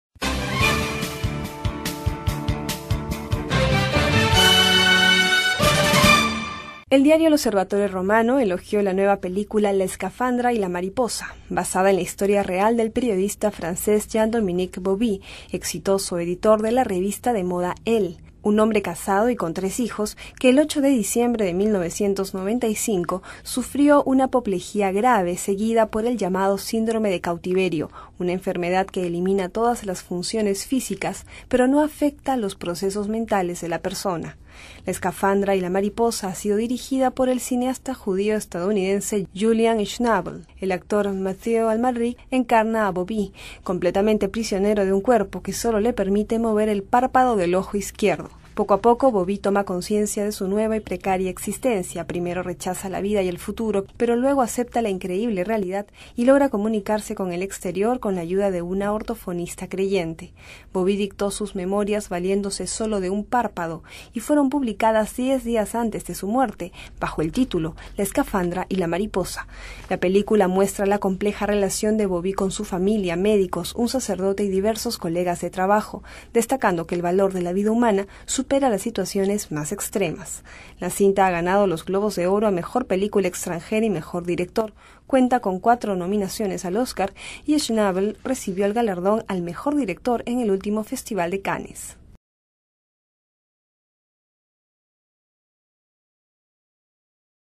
Jean-Dominique Bauby
El diario oficioso del Vaticano, L'Osservatore Romano (LOR) elogió la nueva película "La escafandra y la mariposa" que narra la historia real de un periodista francés que quedó completamente paralizado a la edad de 43 años y logró comunicarse moviendo un párpado.